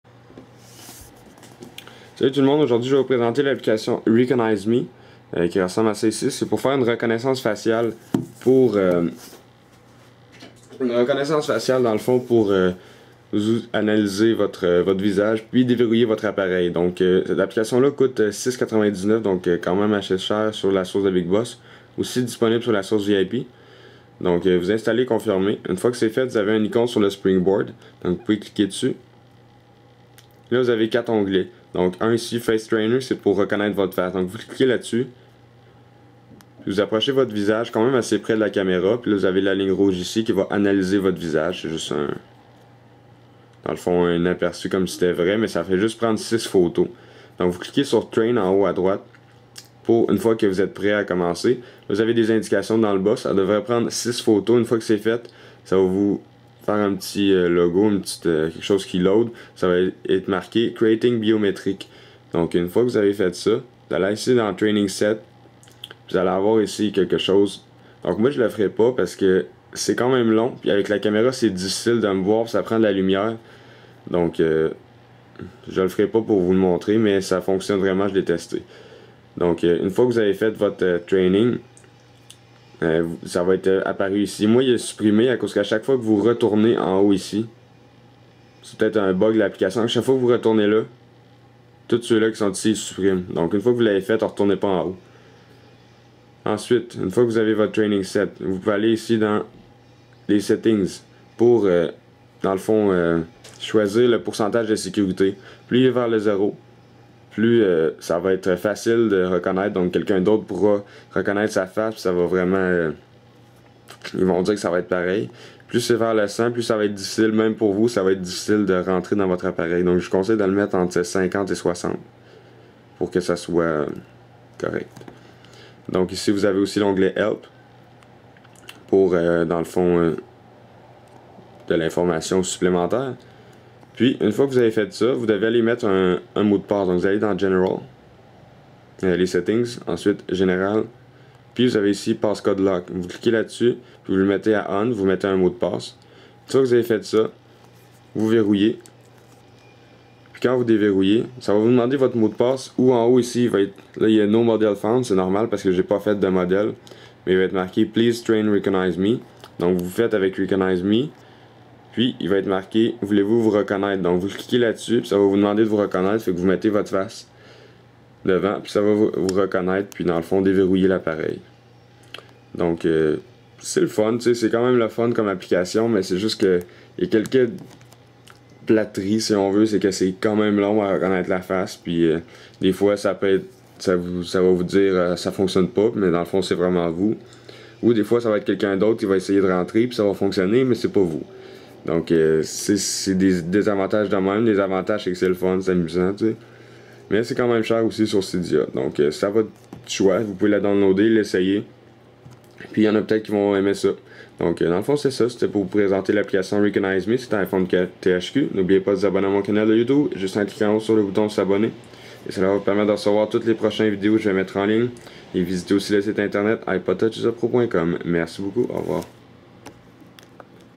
RecognizeMe - Une reconnaissance faciale pour dévérouiller votre appareil
Cette vidéo vouso présente l'application RecognizeMe. Vous pouvez donc dévérouiller votre appareil par reconnaissance faciale.

Application : RecognizeMe (Cydia)

Amicalement,
iPhoneTHQ :)

Merci